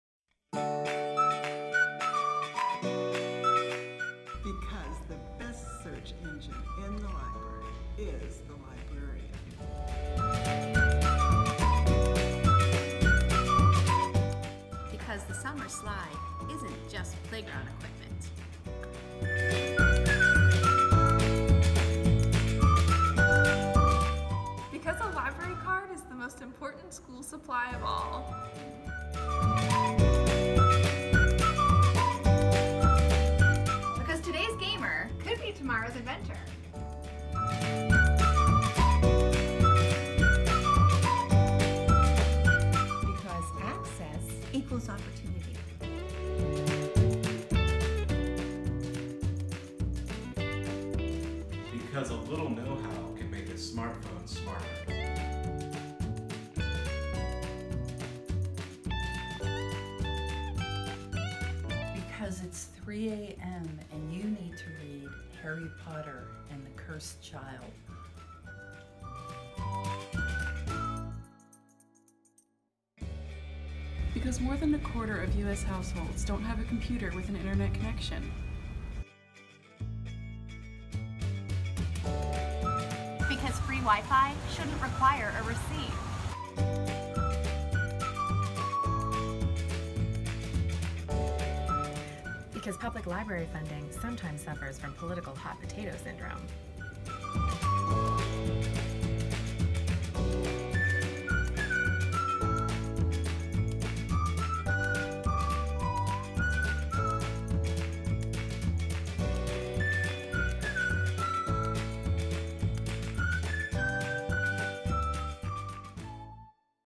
Top 10 Reasons to Support the Sewickley Public Library
There are so many reasons to love and support the Sewickley Public Library! Libraries can open doors to new learning opportunities and can help connect you to the resources you need to achieve your goals. Help sustain the important services that SPL provides by donating today. During the month of September, all gifts are matched by the Jack Buncher Foundation!